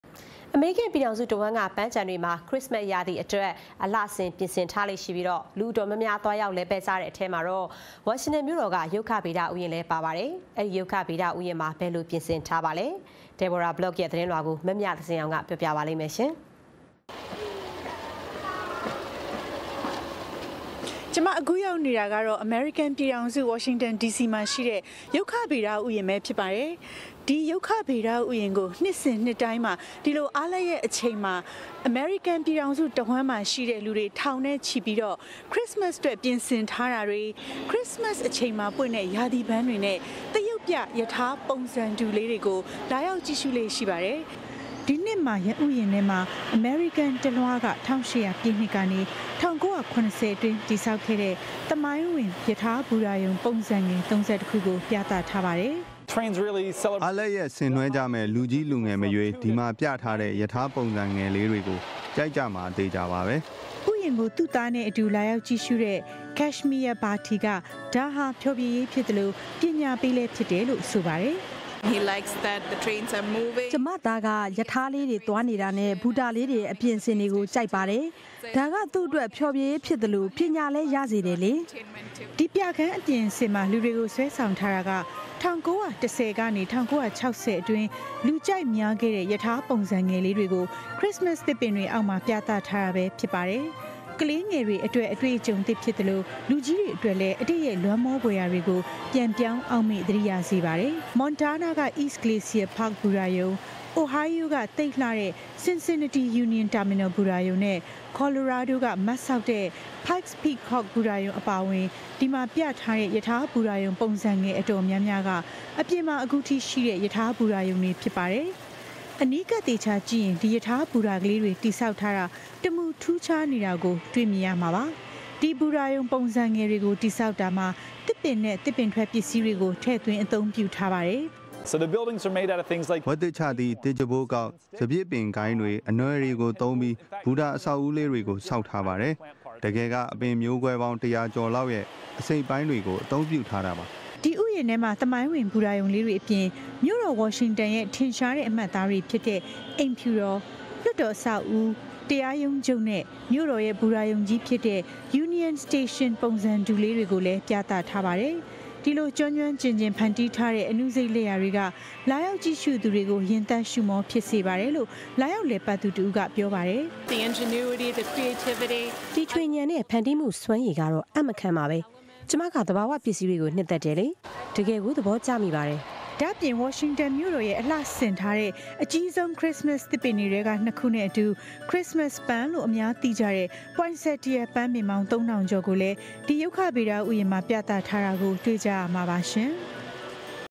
၀ါရွင္တန္ရုကၡေဗဒ ဥယာဥ္ ခရစၥမတ္
အေမရိကန္ျပည္ေထာင္စုတ၀ွမ္းက ပန္းျခံေတြမွာ ခရစၥမတ္ရာသီအတြက္ အလွဆင္၊ ျပင္ဆင္ ထားေလ့ရွိၿပီး လူေတာ္ေတာ္မ်ားမ်ား သြားေရာက္လည္ပတ္ၾကတဲ့အထဲမွာ ၀ါရွင္တန္ၿမိဳ႕ေတာ္က ရုကၡေဗဒ ဥယာဥ္လည္း ပါ၀င္ပါတယ္။ အဲဒီ ရုကၡေဗဒ ဥယာဥ္မွာ ဘယ္လို ျပင္ဆင္ထားပါသလဲ၊ မျမသဇင္ေအာင္ ေျပာျပပါလိမ့္မယ္။